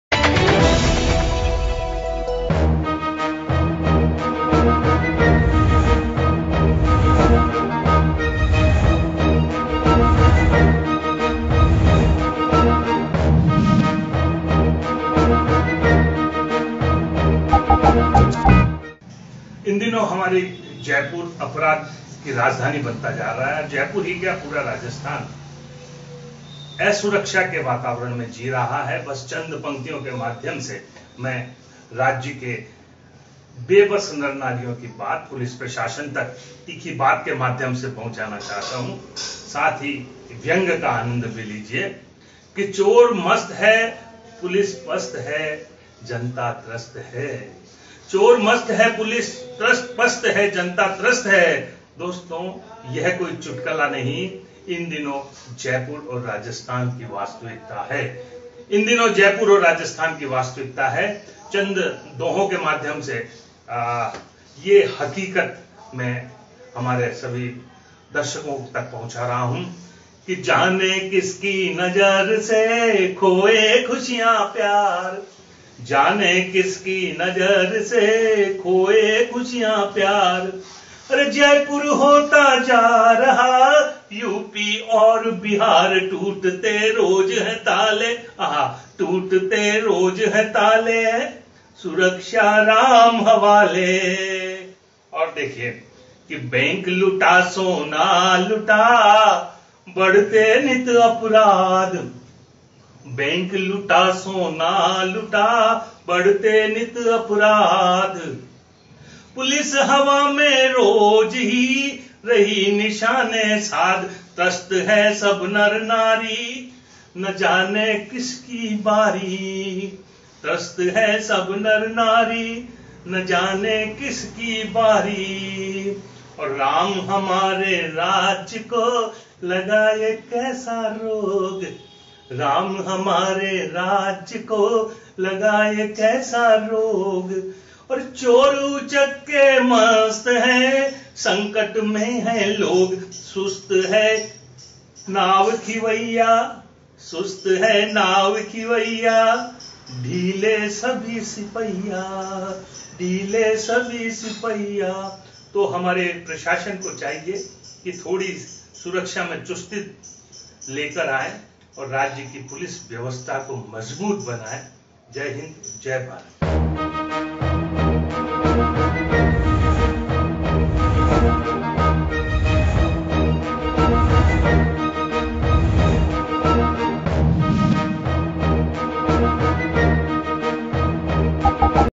Teekhi Baat on "Muthoot Finance robbery" By Kishor Pareek Kishor | तीखी बात
Teekhi Baat on "Muthoot Finance robbery" By Kishor Pareek Kishor
| तीखी बात |